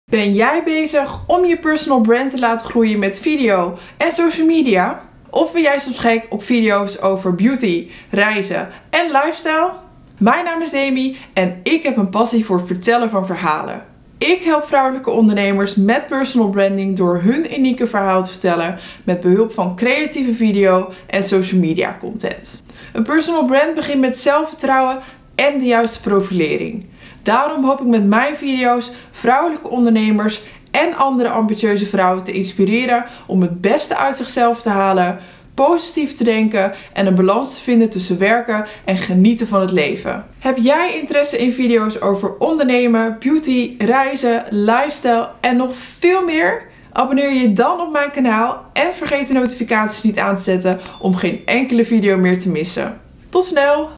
Kanaal trailer: Wie ben ik, wat doe ik, hoe doe ik dat en waarom (2020)
In deze kanaal trailer laat vertel ik wie ik ben, wat ik doe, hoe ik dat doe en waarom. Meer video's zien? Neem dan een kijkje op mijn YouTube kanaal, Instagram of op mijn LinkedIn voor mijn werkervaring.

Mijn naam is Demi Groen en ik ben sinds april 2018 als zelfstandig ondernemer gestart om vrouwelijke ondernemers te helpen met personal branding via video en social media. De video’s die ik maak gaan over ondernemen, personal branding, beauty, reizen en lifestyle. Daarnaast vind ik het ook leuk om tijdens (zakelijke) evenementen of vakanties mijn vlog camera erbij te pakken. Ik heb een enorme passie voor het vertellen van verhalen en het geven van bruikbare tips op video en social media. Ik hoop hiermee vrouwelijke ondernemers en anderen te inspireren en te helpen om uit hun comfortzone te treden, positief te denken en ergens volledig voor te gaan. Ben jij benieuwd hoe je jouw bedrijf kan laten groeien met behulp van online video of social media? Of ben jij soms gek op beauty, reizen of lifestyle? Elke woensdag om 17:00 komt er een nieuwe video online waarin humor, efficiëntie én een eerlijke mening centraal staan. Meer weten? Neem dan een kijkje op mijn Instagram om te zien wat mij dagelijks bezighoud.